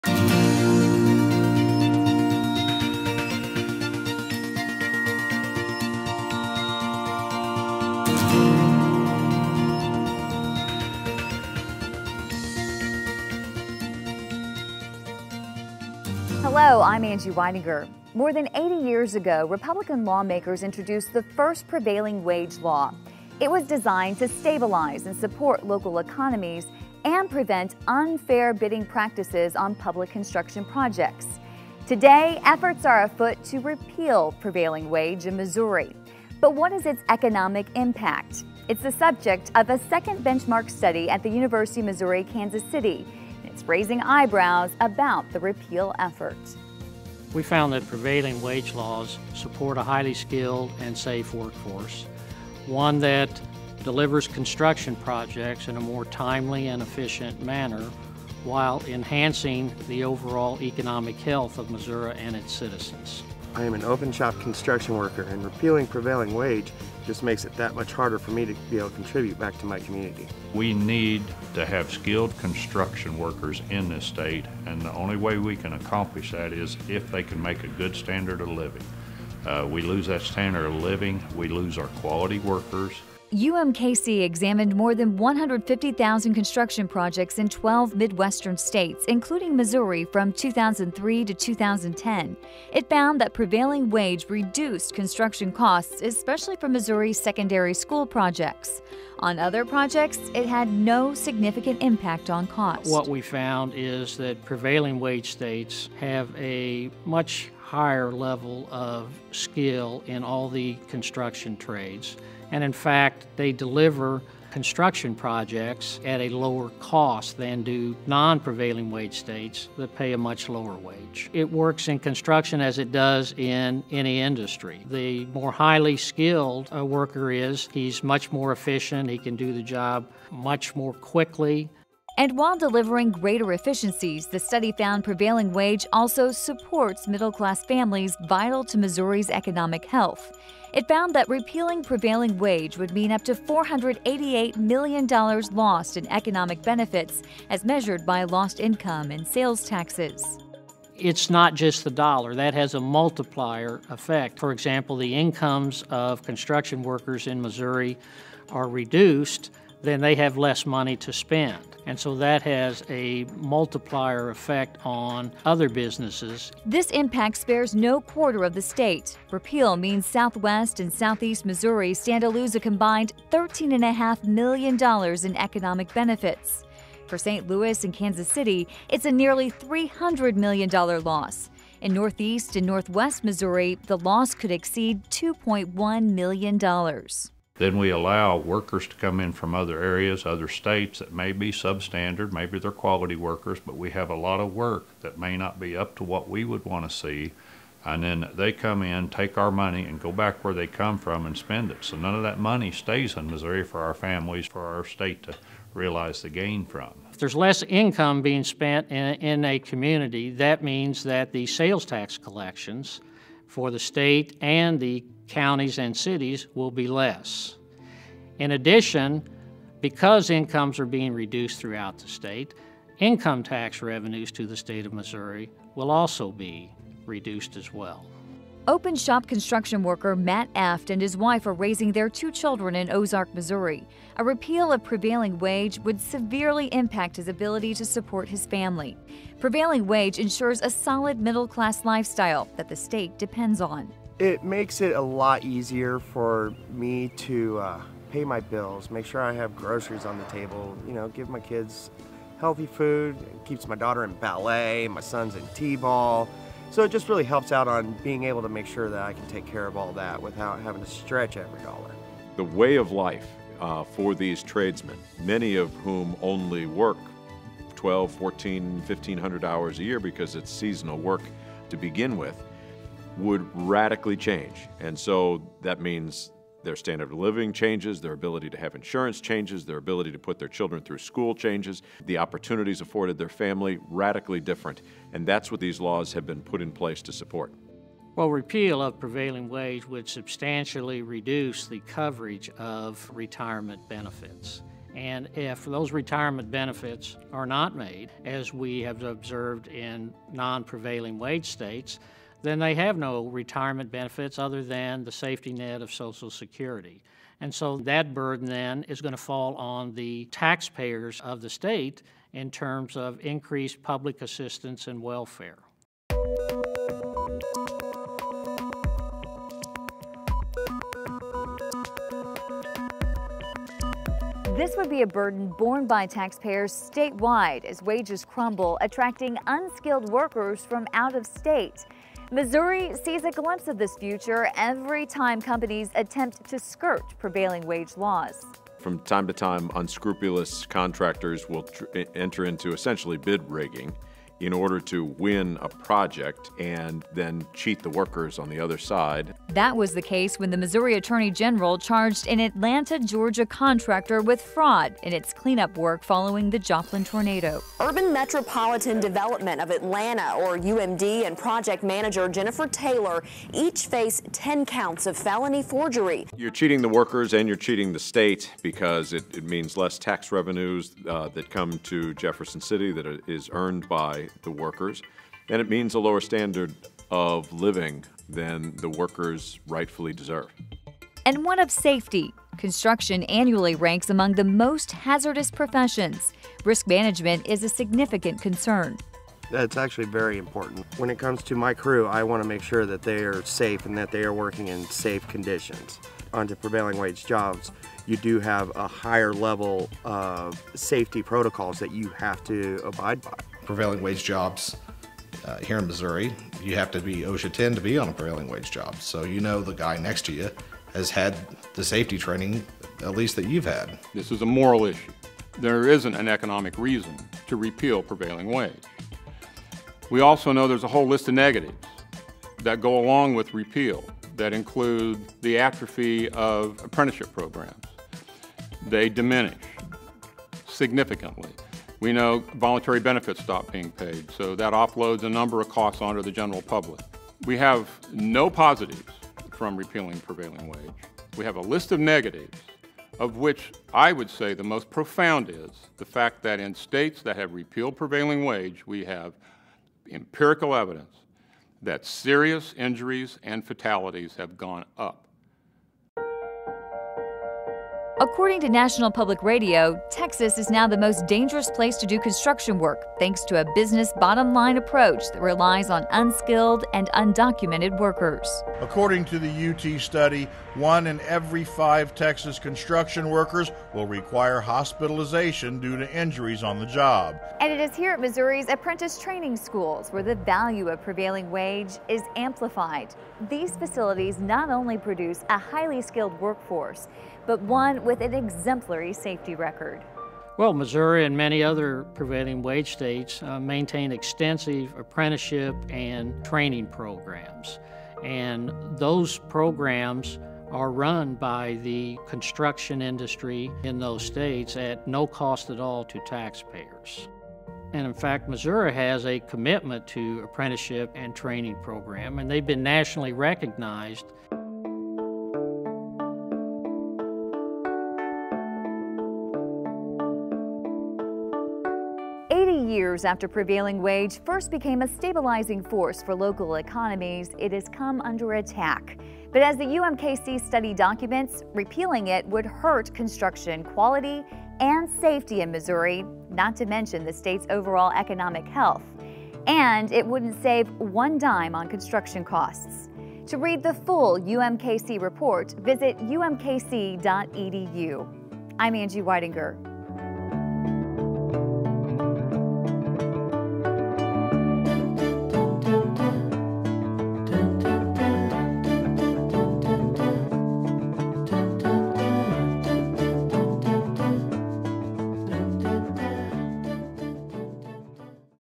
PREVAILING WAGE - AN ECONOMIC IMPACT REPORT BY THE UMKC
Researchers at the University of Missouri - Kansas City detail the results of their prevailing wage study - this may surprise you!